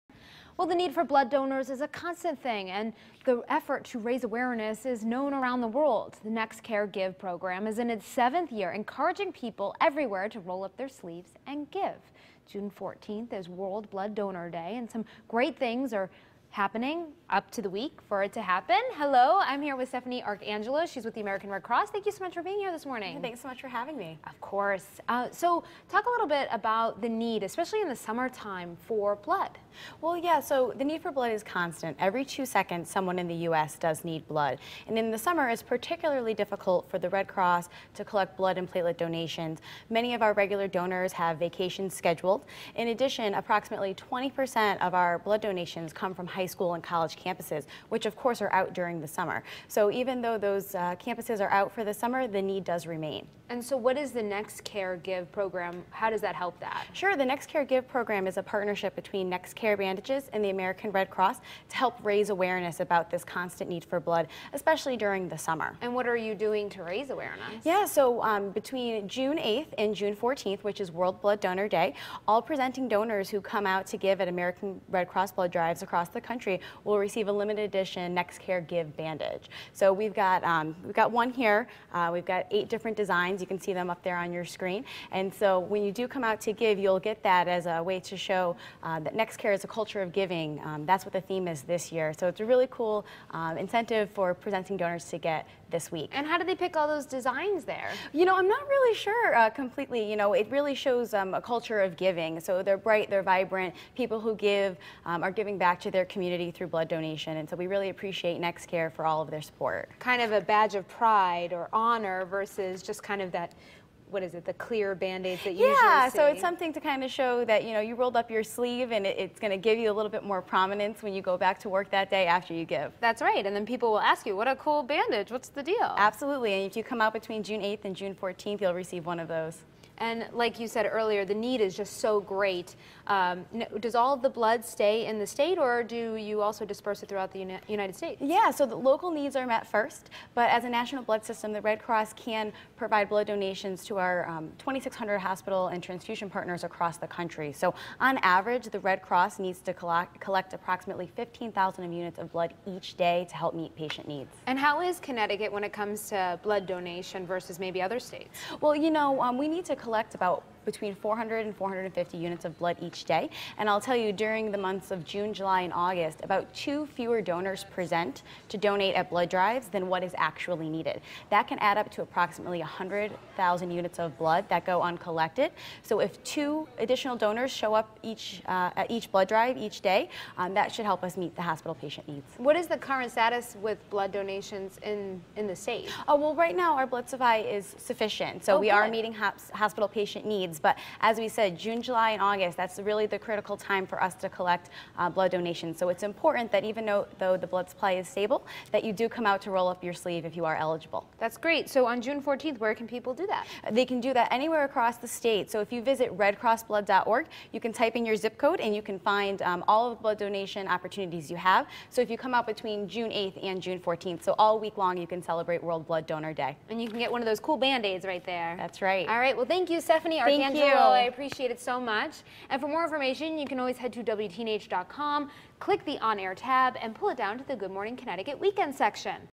World Blood Donor Day
There is a constant need for blood donors, and the effort to raise awareness is known worldwide. The Nexcare Give Program is in its seventh year, encouraging people everywhere to roll up their sleeves and give. June 14th is World Blood Donor Day, and some great things are set up all week leading up to it.